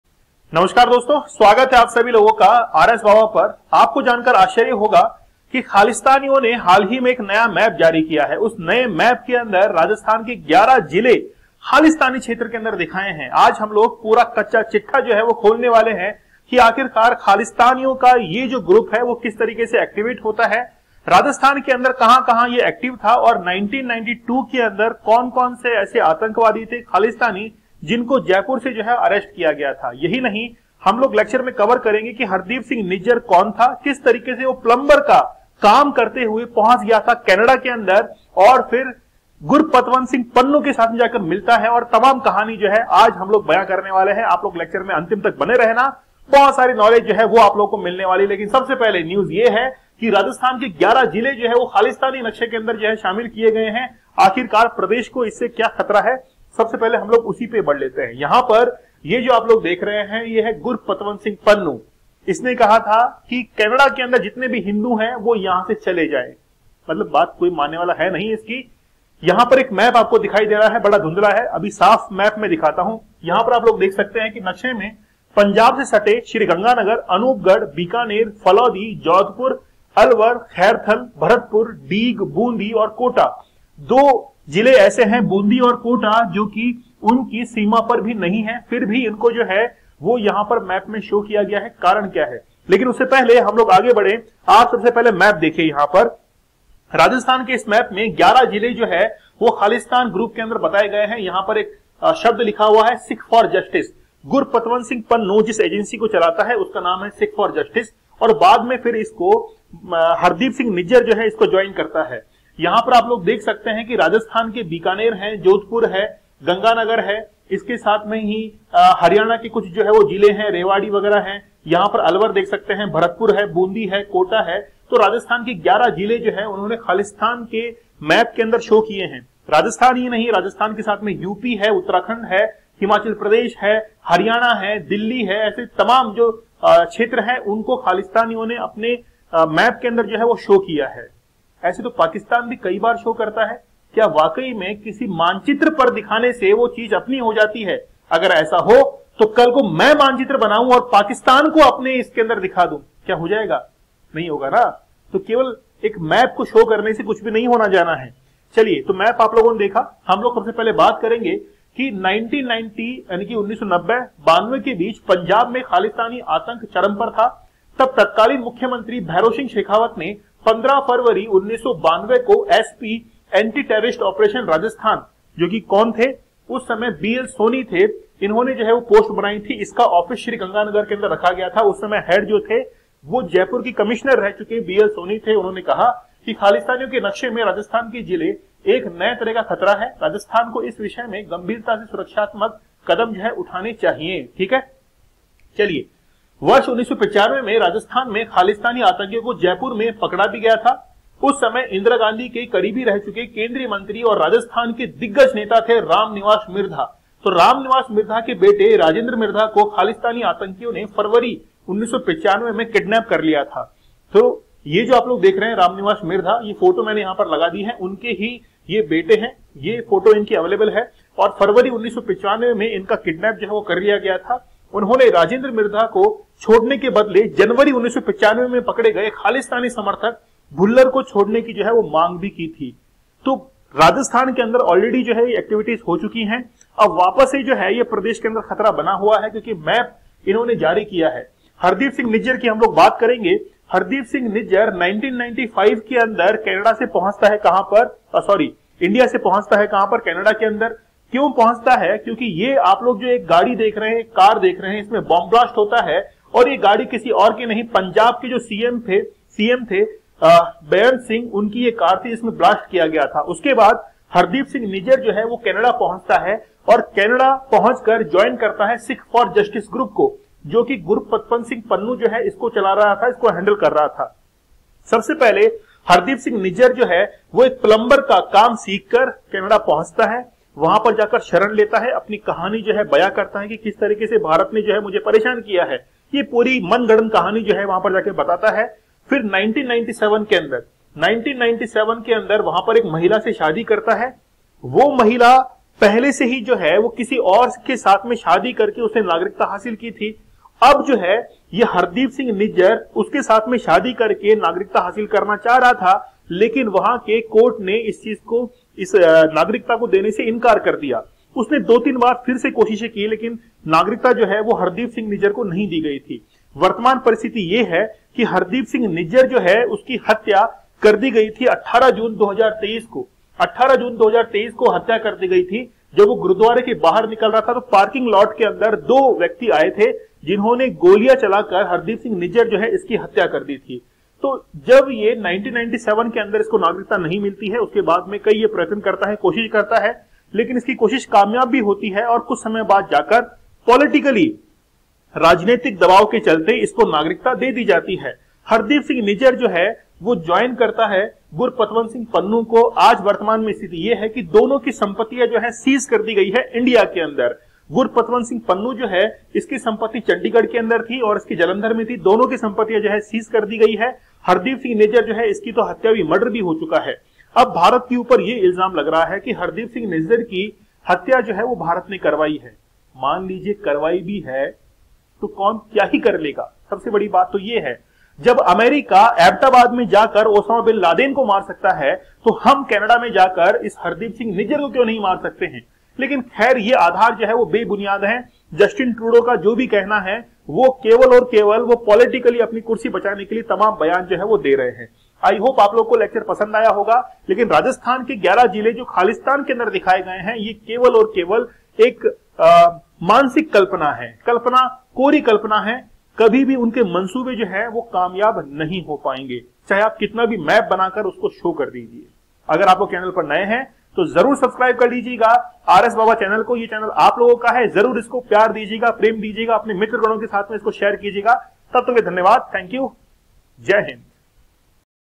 राजस्थान के 11 जिले खालिस्तान के नक्शे में शामिल | Khalistan | Rajasthan Districts in Khalistani Map
As relations between India and Canada continue to deteriorate, the Canadian government appears to be concealing signs of terrorist activities by Khalistani groups operating within the country. A controversial poster calling for the assassination of three Indian diplomats, which was placed outside the Guru Nanak Sikh Gurdwara temple in Surrey, has now been removed. This poster had been erected following the mysterious killing of Hardeep Singh Nijjar outside the Gurudwara.

On September 23rd, a video clip from Global News Canada went viral on social media, showing the removal of the poster that called for the assassination of Indian diplomats outside the Gurdwara in Surrey, Canada. Similar posters had been put up in various locations in Canada following the deaths of several Khalistani terrorists, including Hardeep Singh Nijjar. It is noteworthy that Canada has accused India of involvement in Nijjar's murder.